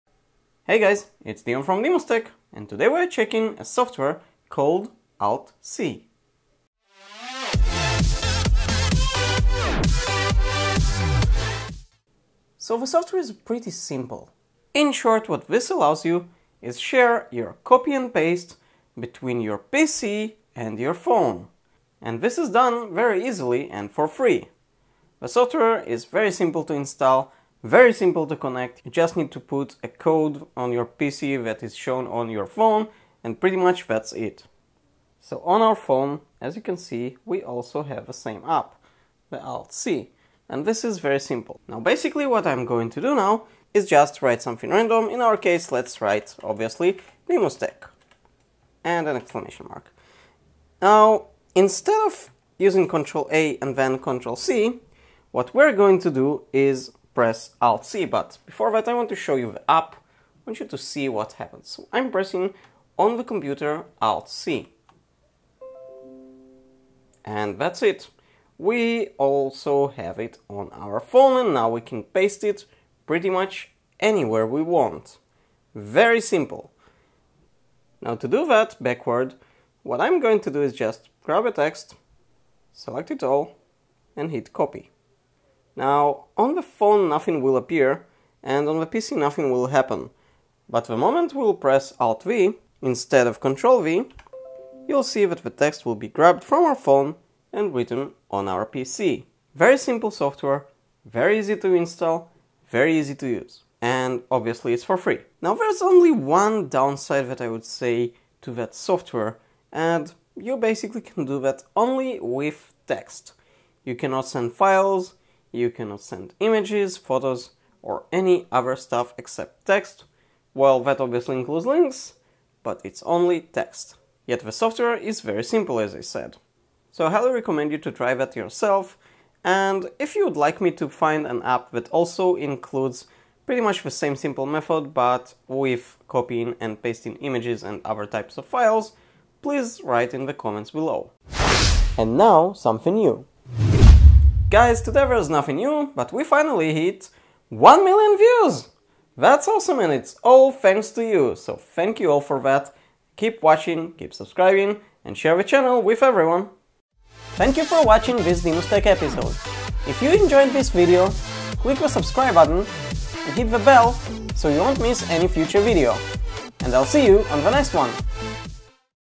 ALT Copy - Copy & Paste seamlessly between PC and Phone
Send easily text and links from your phone to your PC and vice versa very easily with the free ALT-C (ALT Copy) software.
It works very easy and fast.

02:21 - Something New